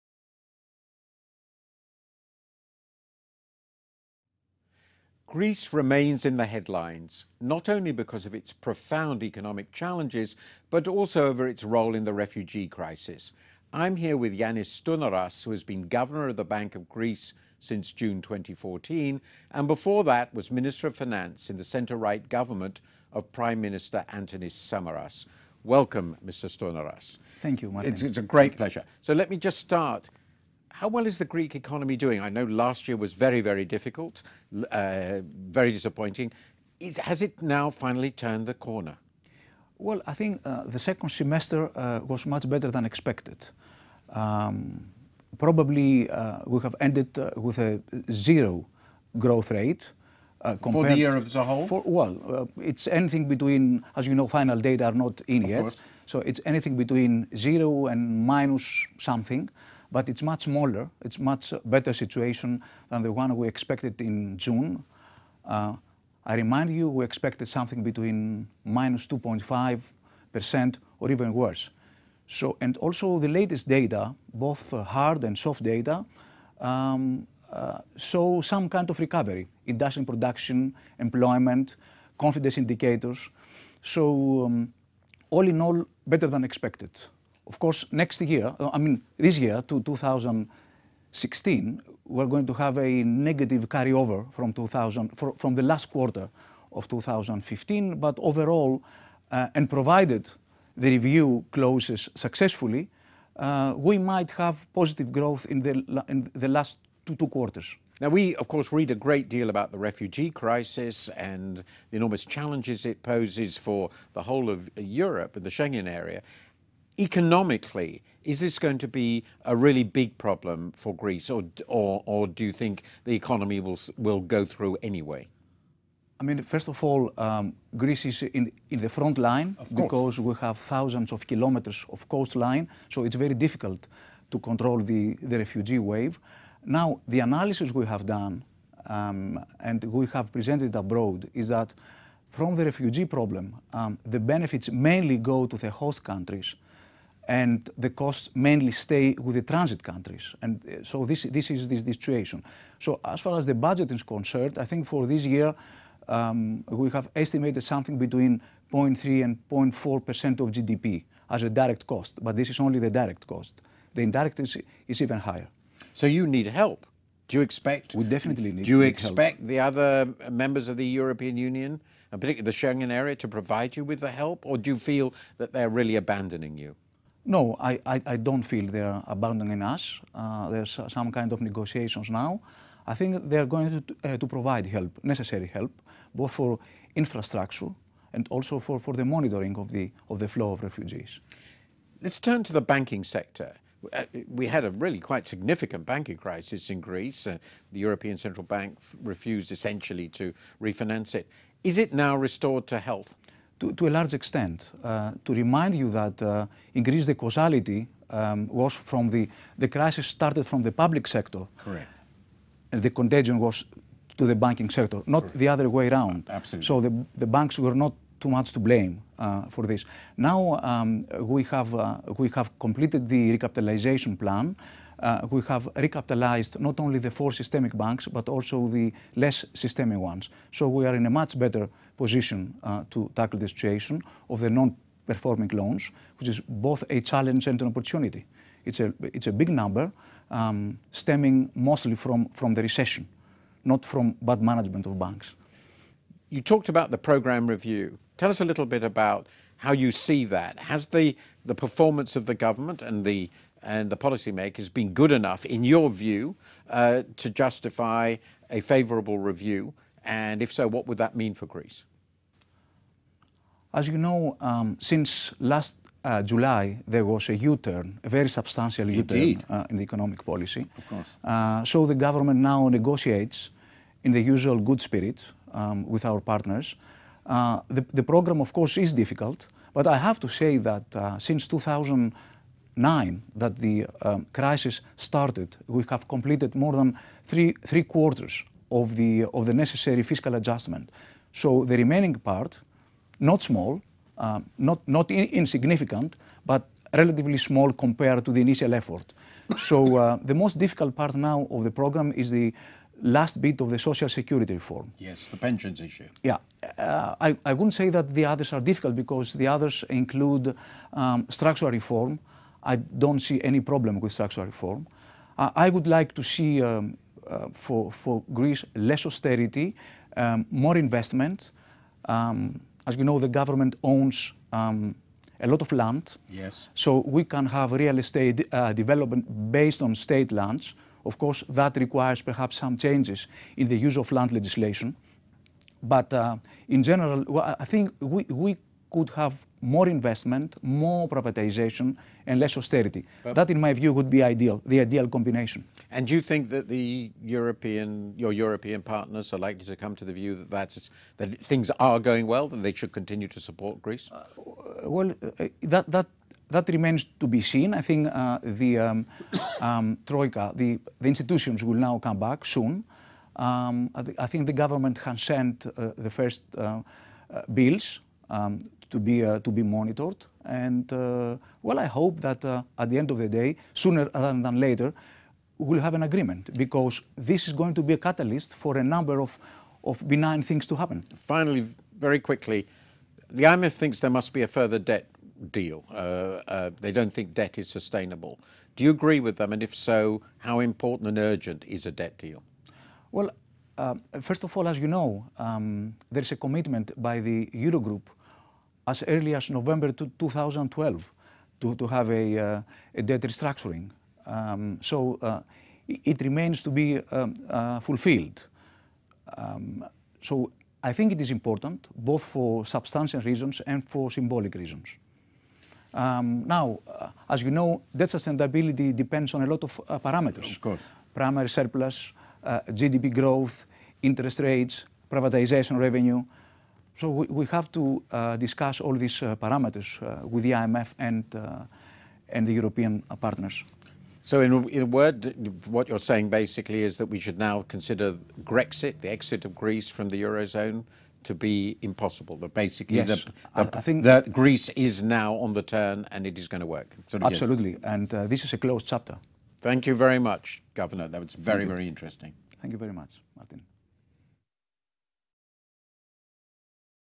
Wolf meets Greece’s central bank chief I FT World
Yannis Stournaras has been governor of the Bank of Greece since June 2014. He talks to Martin Wolf, FT chief economics commentator, about the health of the Greek economy and the strain of the refugee crisis.